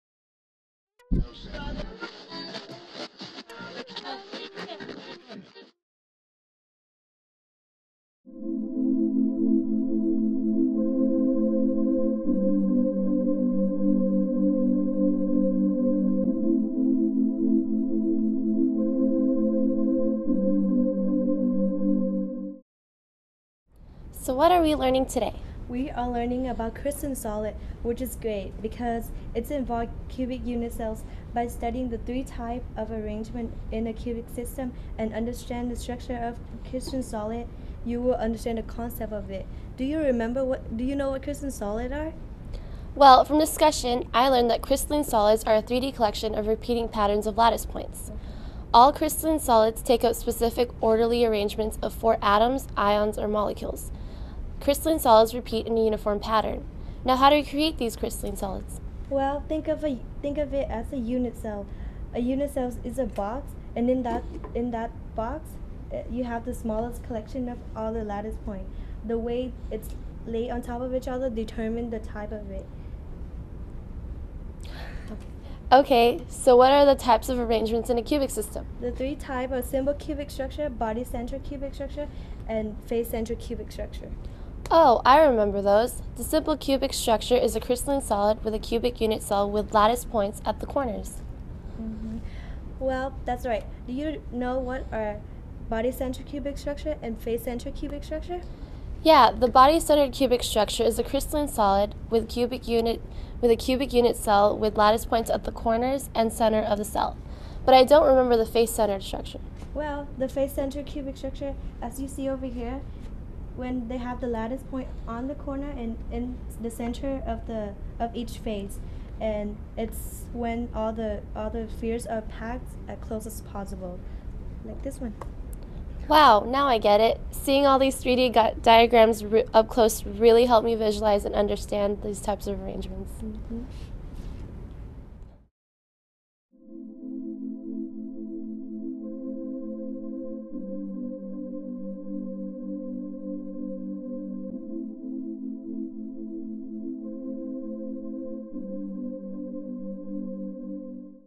Fall 2007 Student Chemistry Video - Crystalline Solids
A student video on Chemistry Laboratory Demonstrations produced by the UCI Teaching, Learning, and Technology Center.